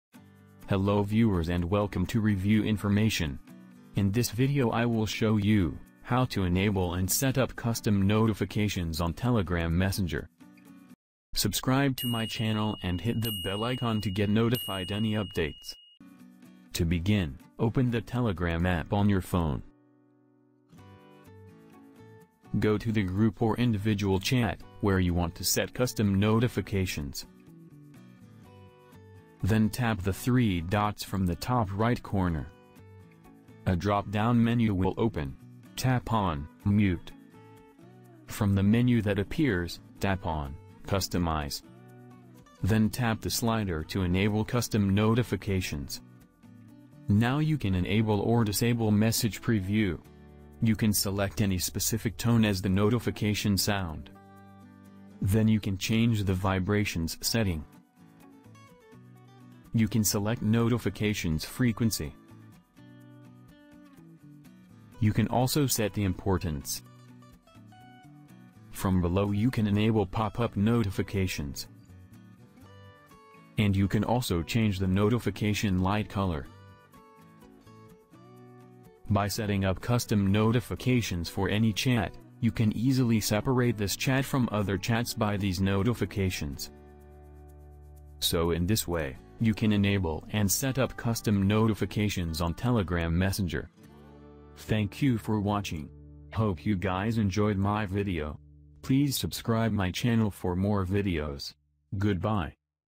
How to Enable and Setup Custom Notifications on Telegram Messenger
In this video I will show you, How to Enable and Setup Custom Notifications on Telegram Messenger.

 Open the Telegram app on your phone. Go to the group or individual chat where you want to set custom notifications. Then tap the three dots from the top right corner. A drop down menu will open. Tap on mute. From the menu that appears, tap on customize. Then tap the slider to enable custom notifications.

Now you can enable or disable message preview. You can select any specific tone as the notification sound. Then you can change the vibrations setting. You can select notifications frequency. You can also set the importance. From below you can enable popup notifications.

And you can also change the notification light color. By setting up custom notifications for any chat, you can easily separate this chat from other chats by these notifications.